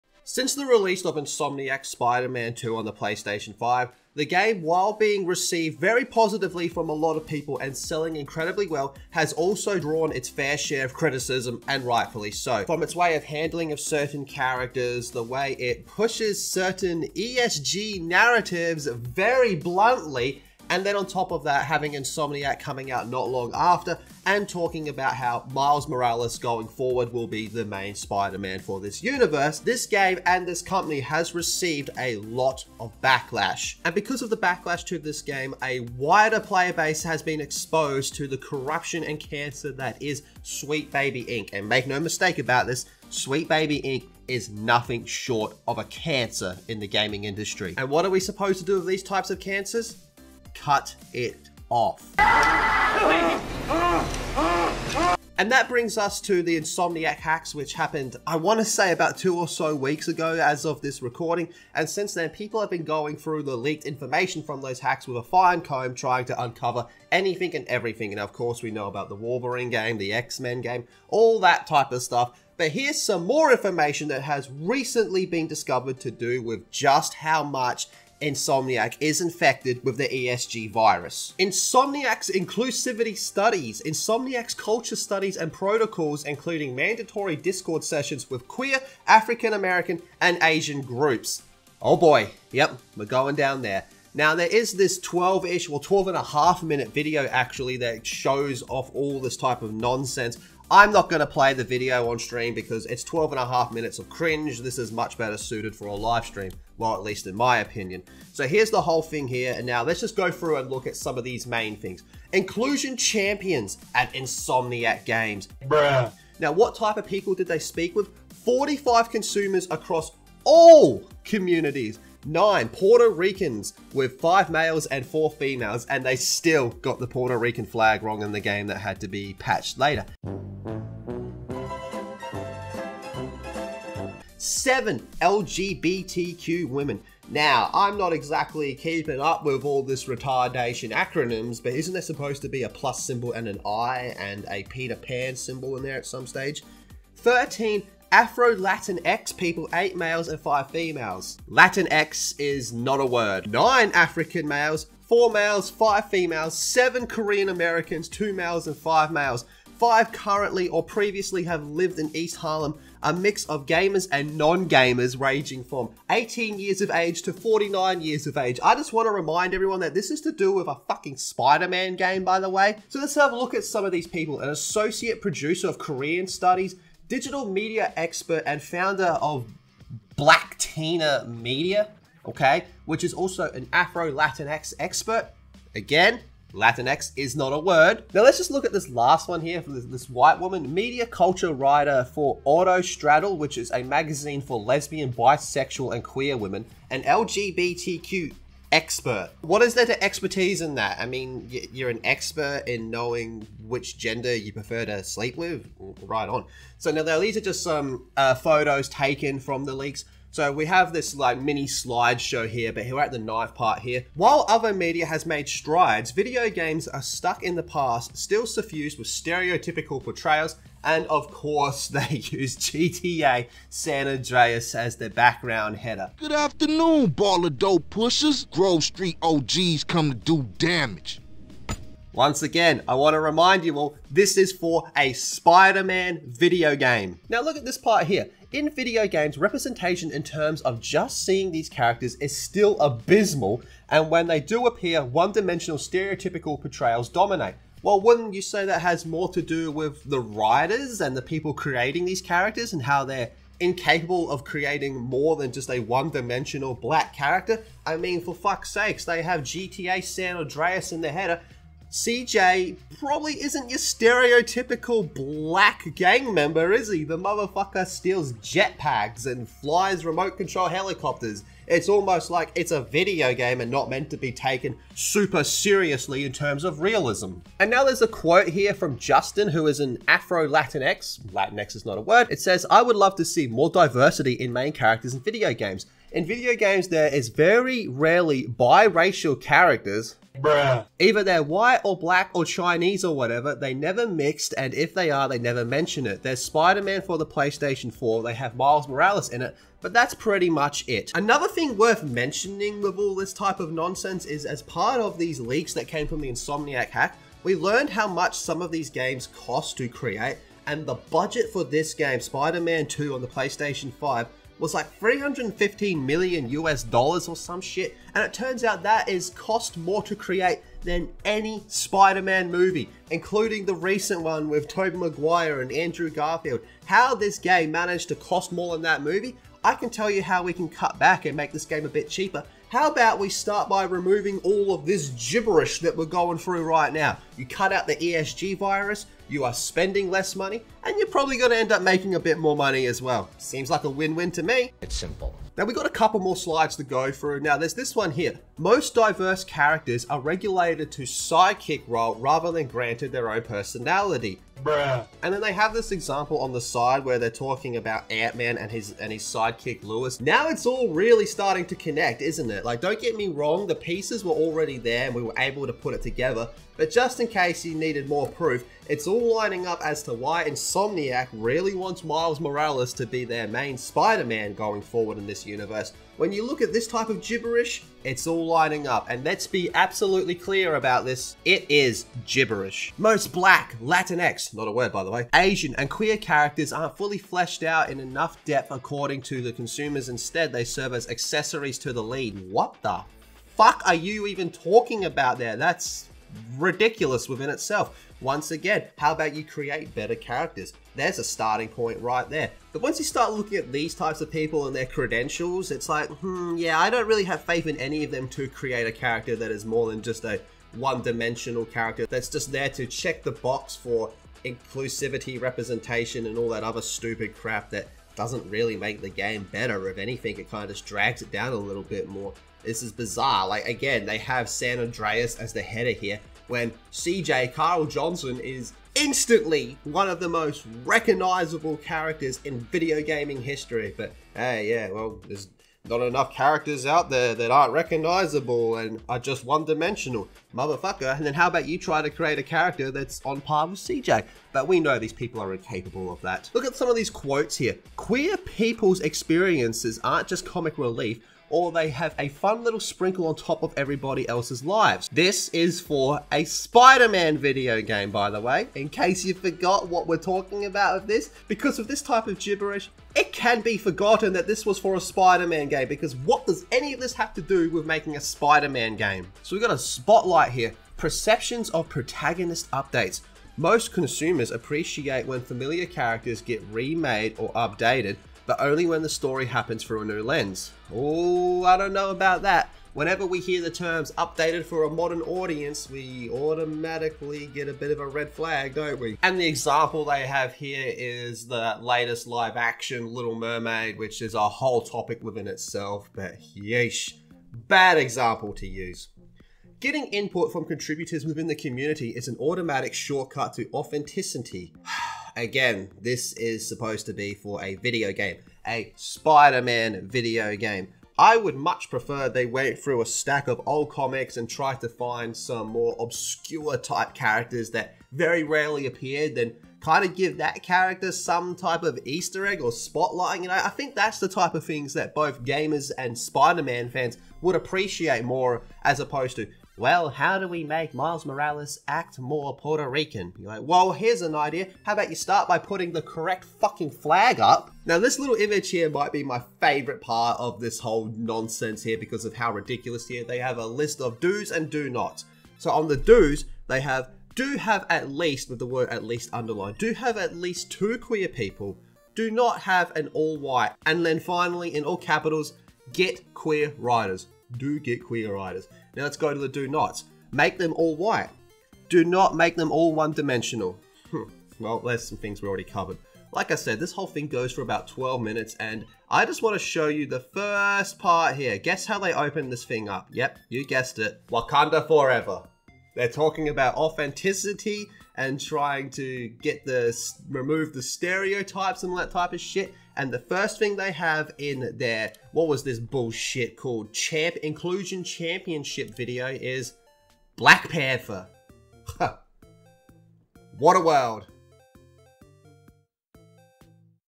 Insomniac Games Is Even Worse Than You Thought
While it's no secret that Insomniac Games has had its fair share of controversies, this deep dive uncovers some shocking revelations about the company's inner workings and practices. From alleged workplace issues to questionable decisions, this video explores the darker side of one of gaming's most acclaimed studios. Join us as we delve into the untold stories and shed light on why some believe Insomniac Games might be worse than what meets the eye. Strap in for an eye-opening analysis that might change the way you view this beloved game developer.